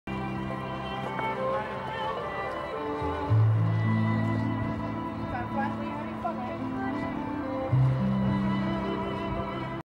Vuurkunstenaares Petra Dankers
opening sauna de Bronsbergen te Zutphen, het uitbeelden van de vier elementen